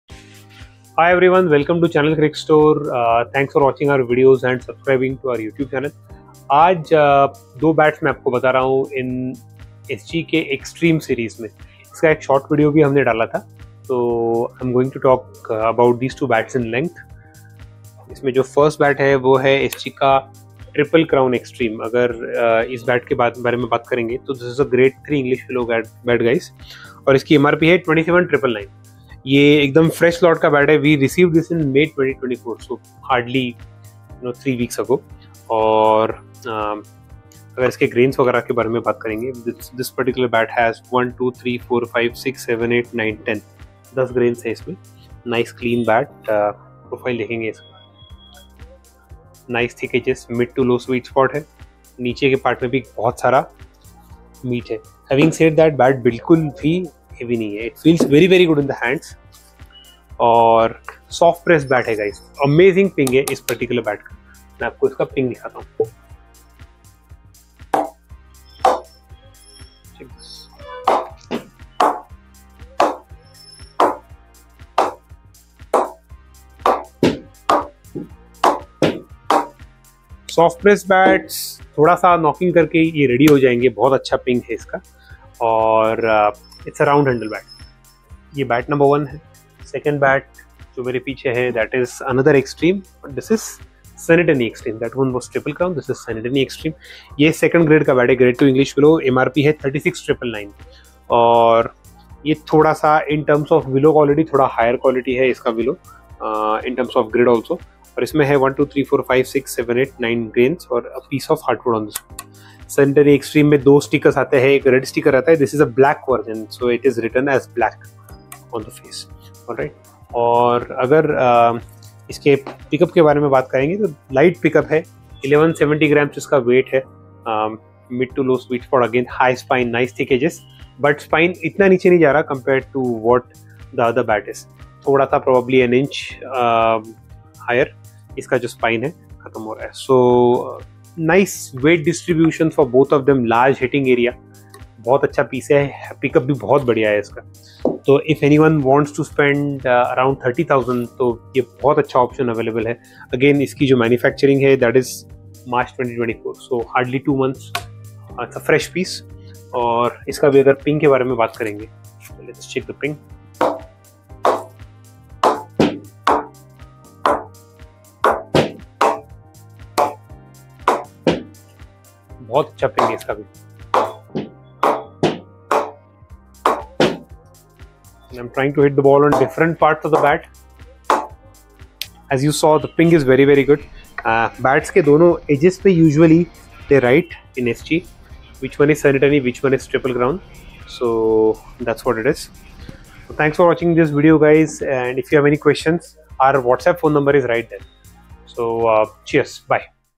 Two Extremely Popular SG Bats in Mid range - SG Triple Crown Extreme and SG Sunny Tonny Extreme
Two Extremely Popular SG Bats in Mid range - SG Triple Crown Extreme and SG Sunny Tonny Extreme are showcased in this video

SG Triple Crown Extreme is a grade 3 English Willow Cricket Bat with MRP 27999
SG Sunny Tonny Extreme is a grade 2 English Willow Cricket Bat with MRP 36999

Both amazing pieces are available at our Gachibowli store in Hyderabad.